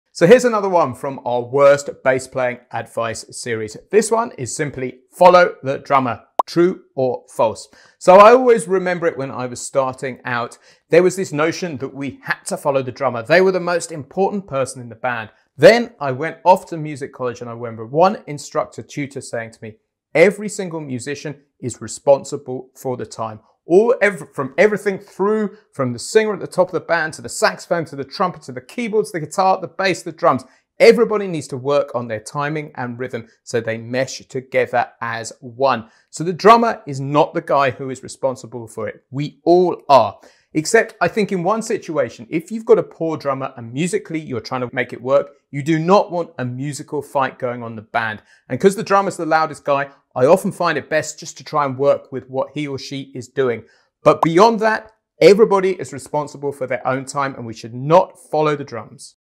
FOLLOW the DRUMMER! true✅ or false❌?
In this video, James explains the common notion of always following the drummer in a band as a bass player.